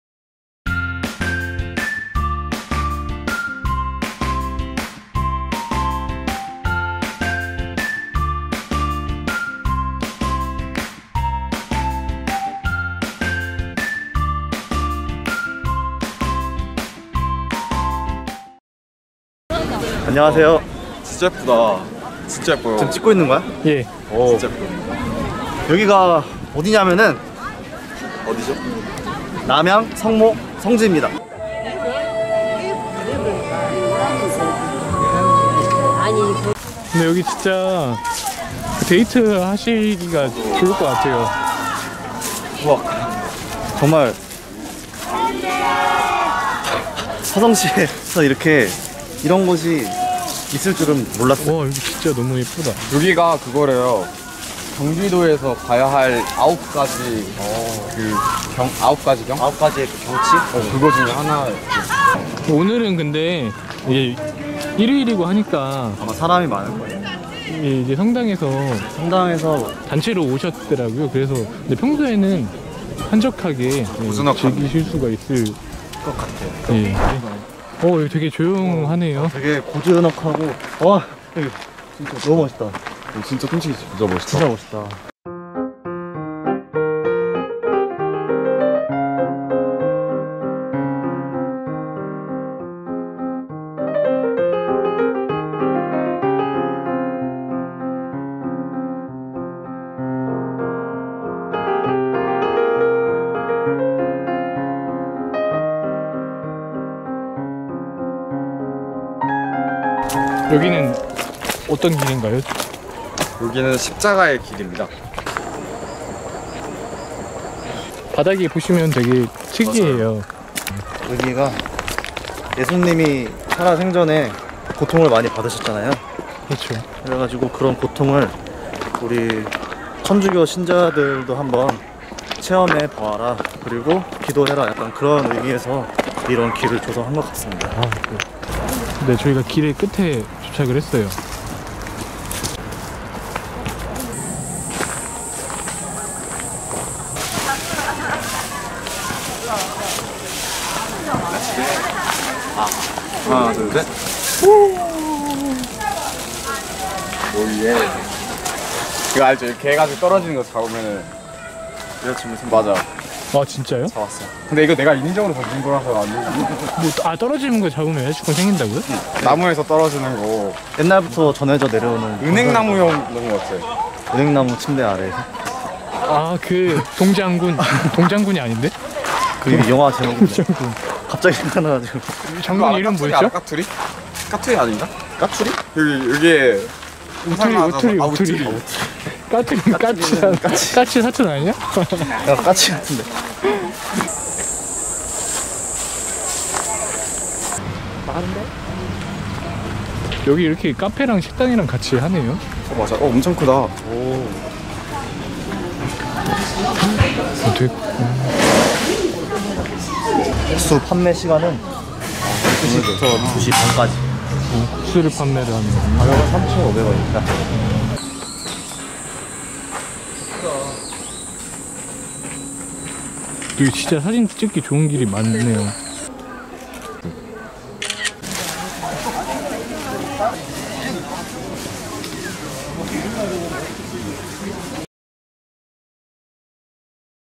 화성탐사대 5화 : 남양성모성지 탐사하기
화성탐사대가 남양성모성지에 떴다!
푸른 가을날, 서포터즈들이 다녀온 남양성모성지 탐사기!